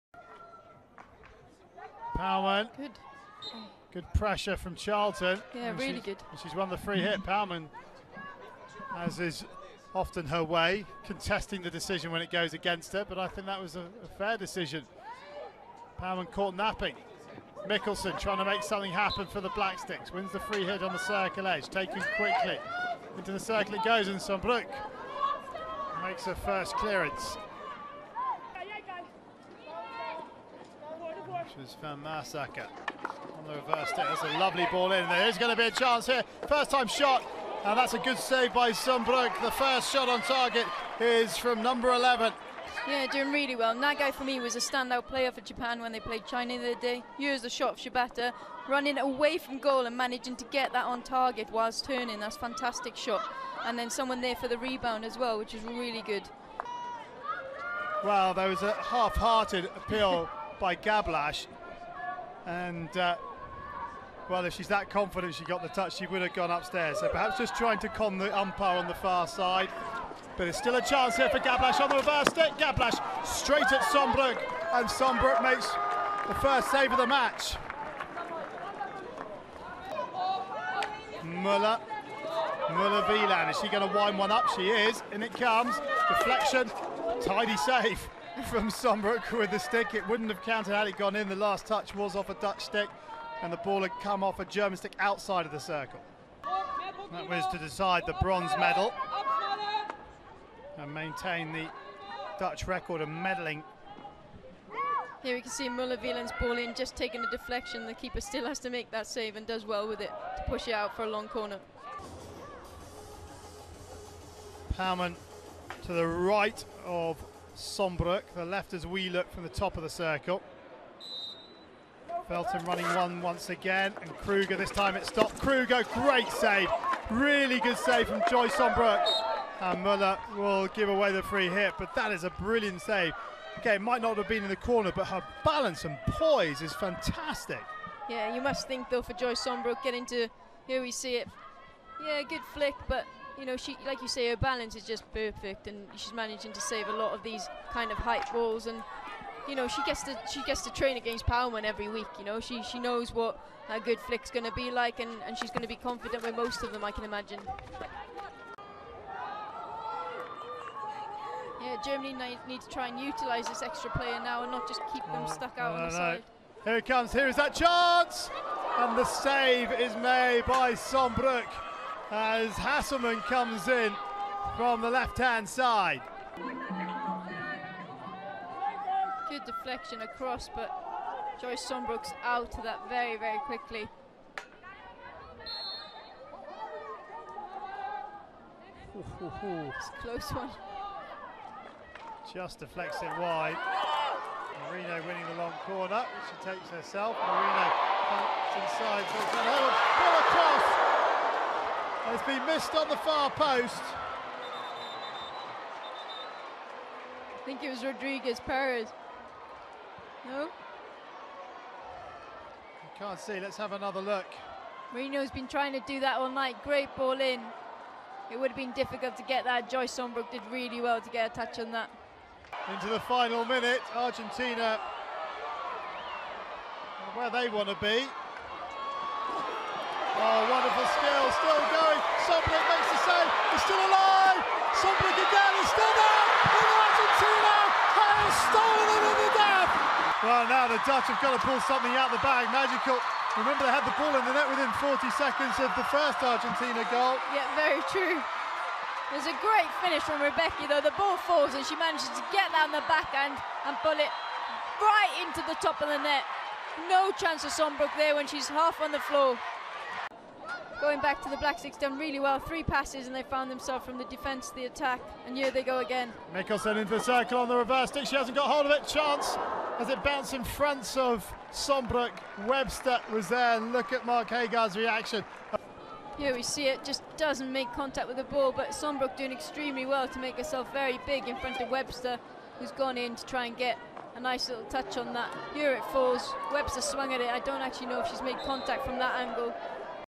hockey joyce sombroek saves Champions Trophy 2014
This are the saves from joyce sombroek . She play's in the dutch hockey team. Her saves are from the Champions Trophy 2014